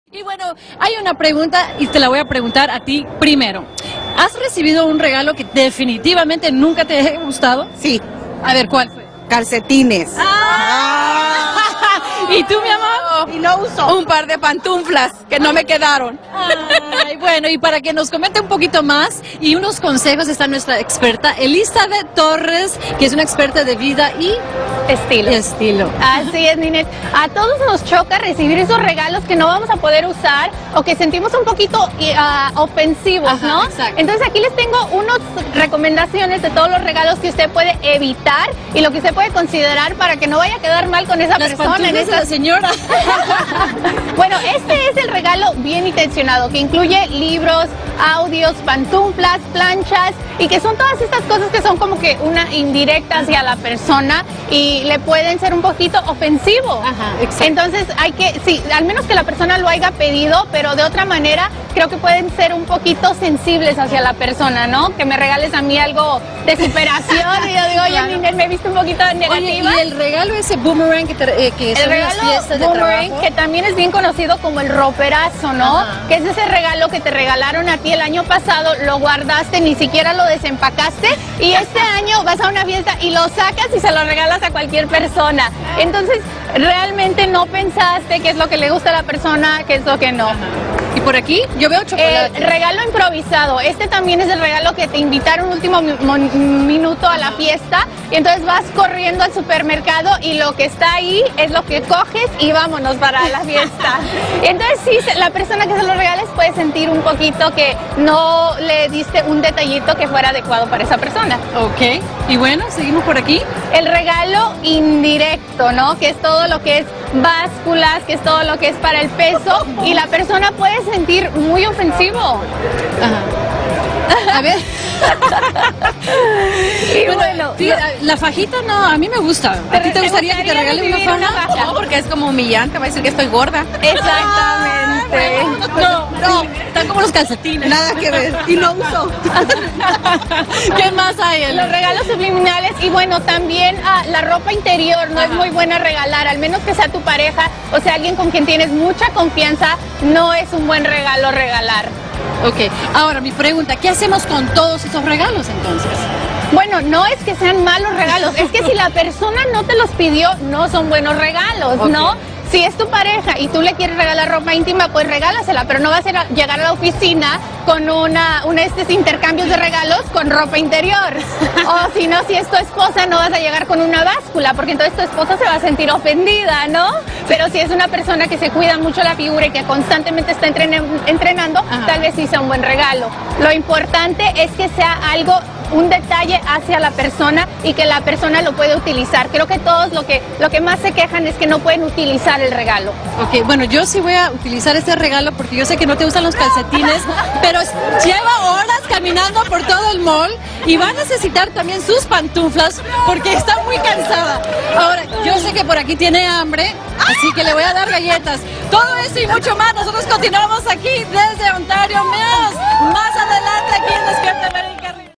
¡Aléjate de los regalos más odiados en las fiestas!
Elizabeth Torres, experta en vida y estilo, habló de todos esos regalos que debes sacar de tu lista de compras.